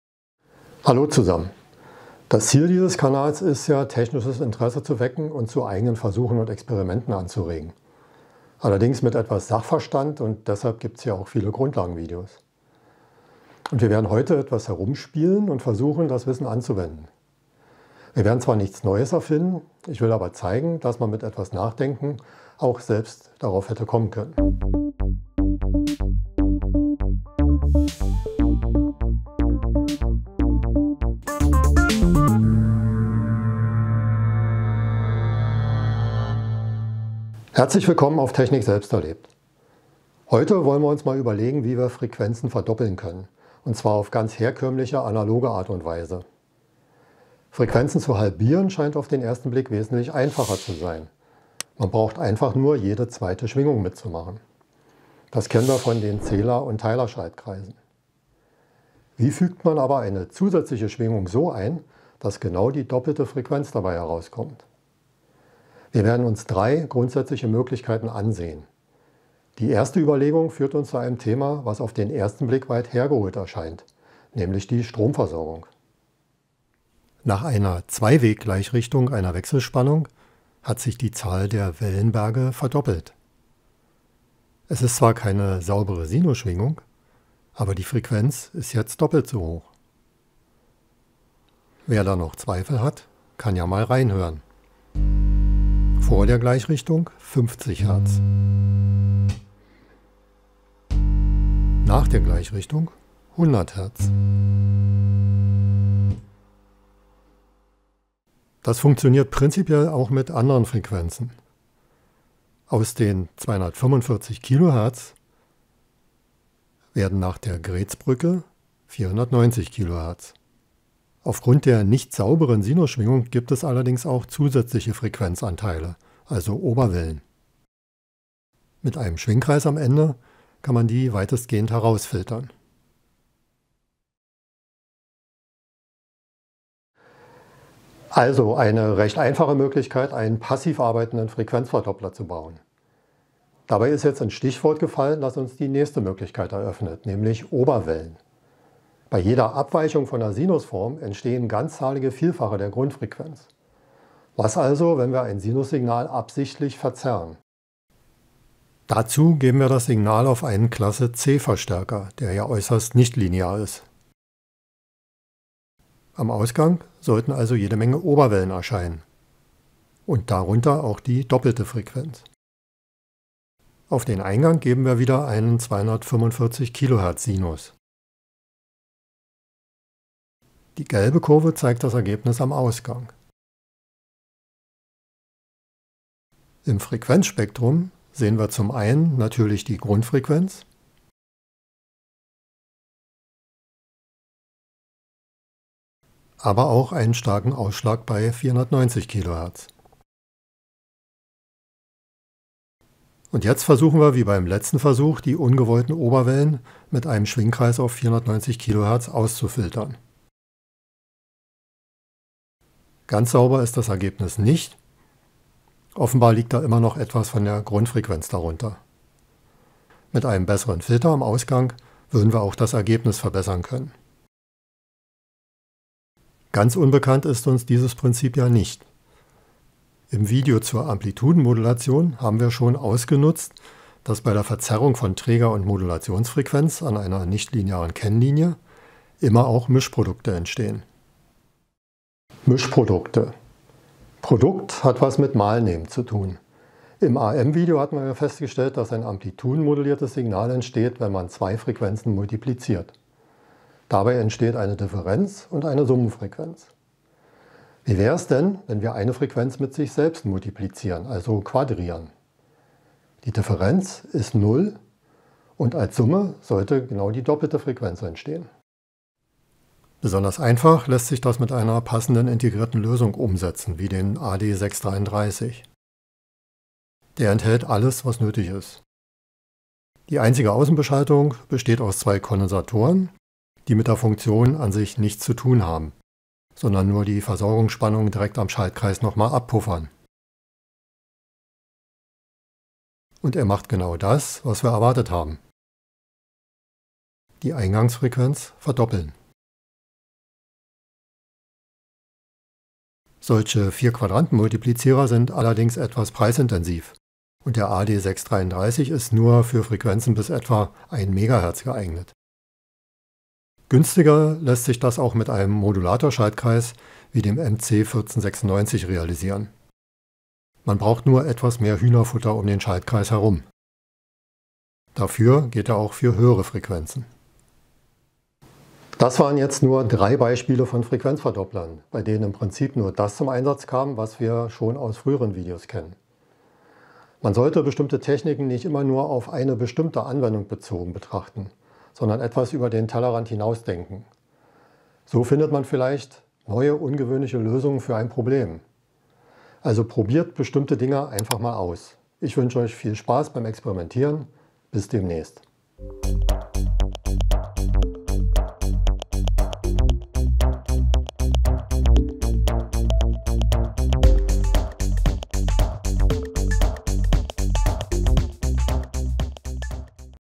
Wie verdoppelt man Frequenzen - Drei Versuche zur analogen Frequenzverdopplung
Die Theorie in die Praxis umgesetzt.
Drei gängige Möglichkeiten zur Frequenzverdopplung, auf die man auch hätte selbst kommen können.

0:00 - Worum geht's?
1:16 - Vorbild Stromversorgung
2:28 - Erzeugung von Oberwellen
4:03 - Überlagerung von Frequenzen
5:28 - Fazit